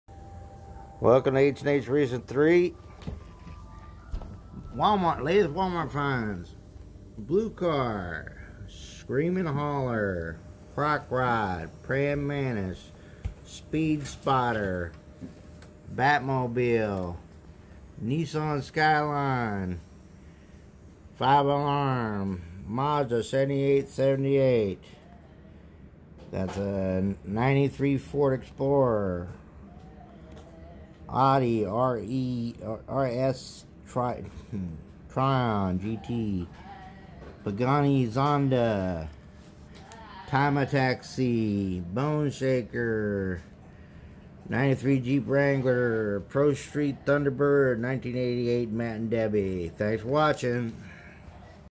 Latest walmart finds what a find at the Rock and HandHracing3#hotwheels oh no its godzilla!
Welcome to Rockingham Oklahoma
 and to Hot wheels vs Matchbox vs Adventure Force and old hot wheels cars. All kinds off racing loopdeloop,speed,distance,jumps anything fun you can do with hotwheels

Hot Wheels Matt and Debbie Hay's 1988 Pro Street Thunderbird
Hot Wheels Zamac Bone Shaker, Dream Garage 4/5
Hot Wheels Pagani Zonda R
Hot Wheels Batmobile, Batman 3/5
Hot Wheels Nissan Skyline GT-R, HW J-Imports 5/10
Hot Wheels 2022 - Audi RS E-TRON GT - Red - HW Green Speed 5/5
Hot Wheels 2022 - 5 Alarm - Pink Fire Truck - HW Rescue 8/10
Hot Wheels Mazda 787B, Retro Racers 4/10
Matchbox Jurassic World '93 Ford Explorer #4
Matchbox '93 Jeep Wrangler #10 Jurassic World Diecast
Hot Wheels 2019 Street Beasts Speed Spider (Spider Car) 210/250, Orange